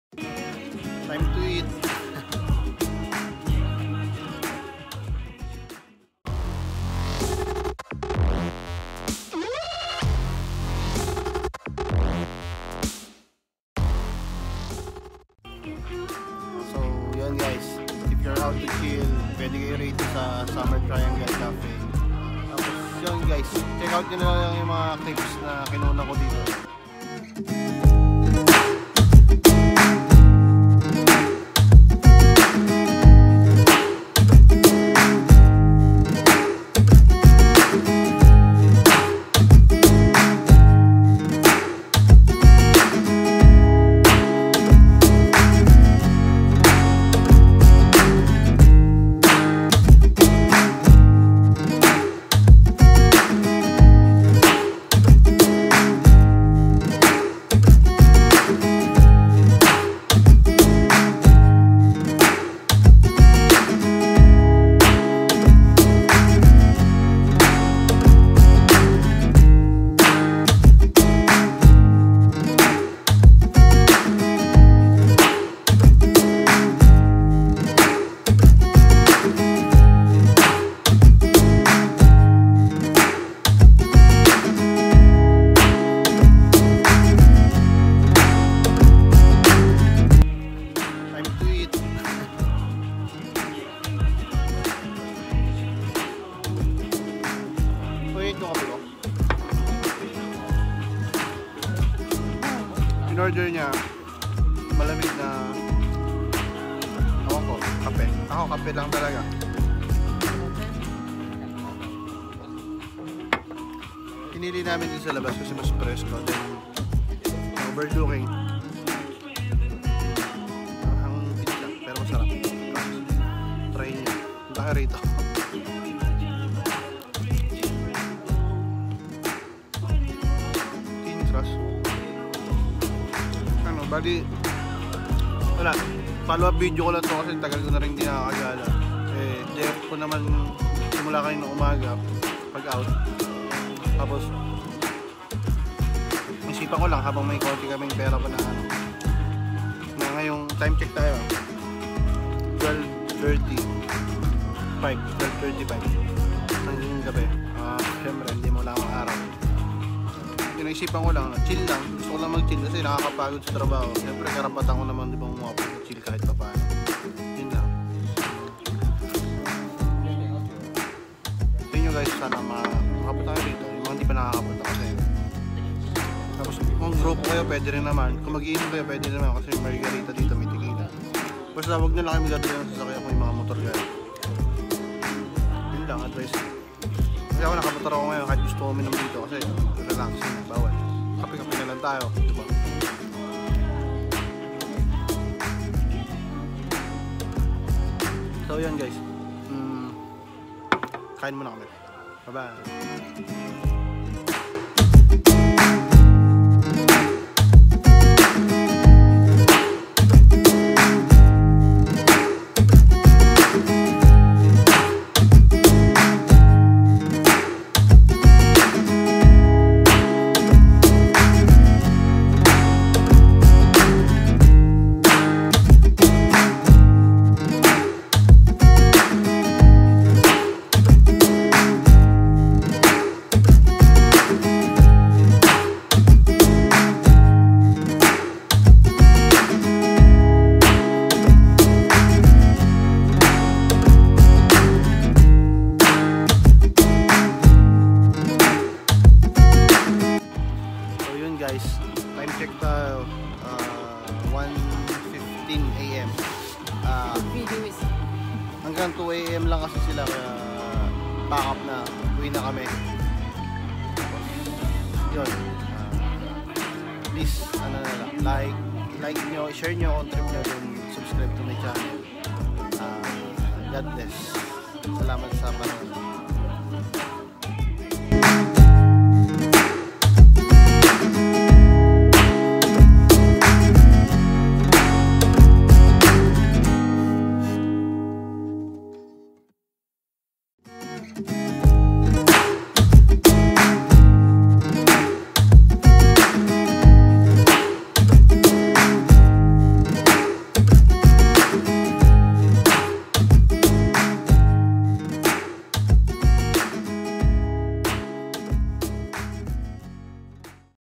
SUMMER TRIANGLE CAFE
--- THIS IS THE PLACE SPECIALLY FOR THE LOVERS TO MEDITATE AND HAVE SOME BABE TIME ^_^

-- EVEN FOR BARKADAS YOU CAN CHILL OUT HERE AND DO YOUR SMALL TALKS WHILE TAKING PICTURES ON DIFFERENT SPOT :)